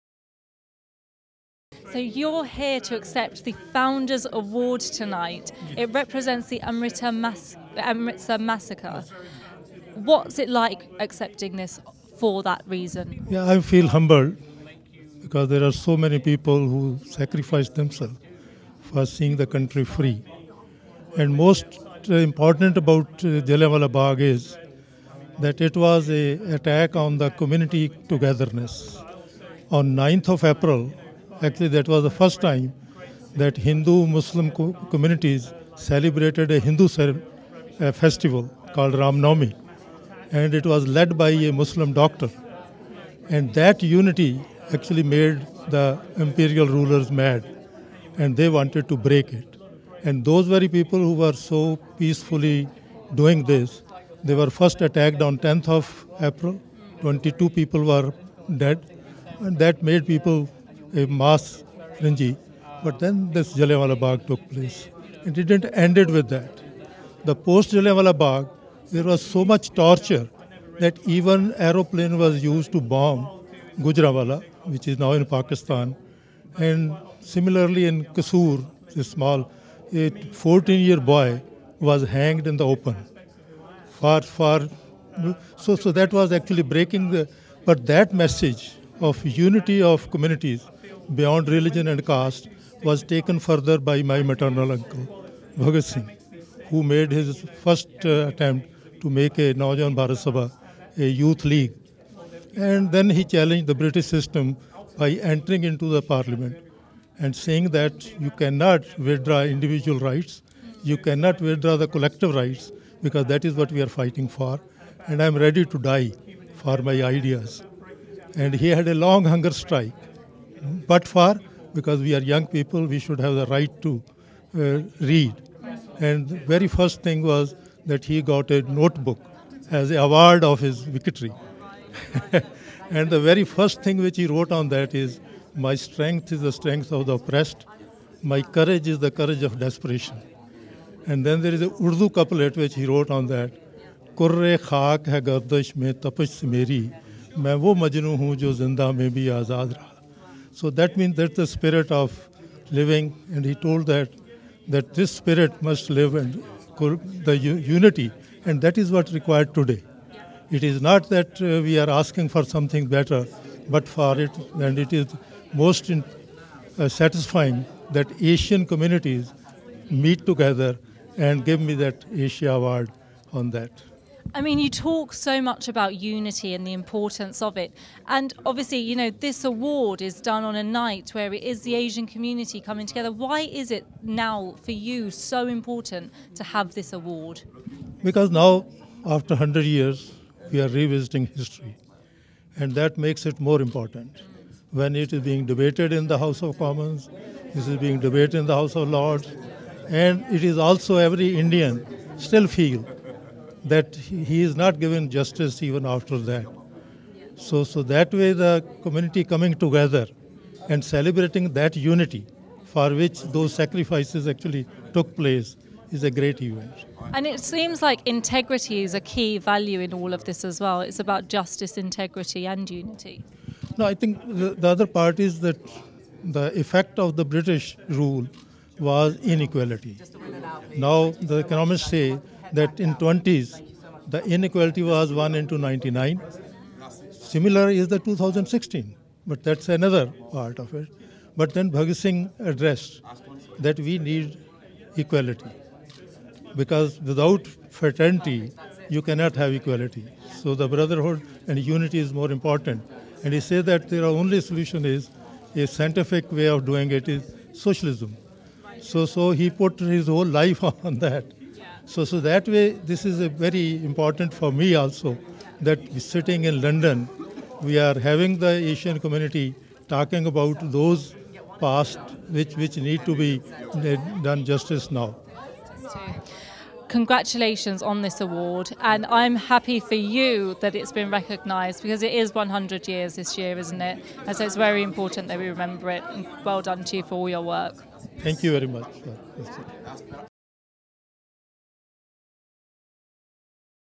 The 9th Asian Awards - Press Room - Jagmohan Singh - Nephew of Bhagat Singh
The 9th Asian Awards - April 12th 2019, The Grosvenor House, London. All the interviews from the Press Room. Interviews by Anila Chowdhry. Jagmohan Singh is the nephew of the late Bhagat Singh (Indian Freedom Fighter) and collected of the Founders award on behalf of the victims of Jalianwala Bagh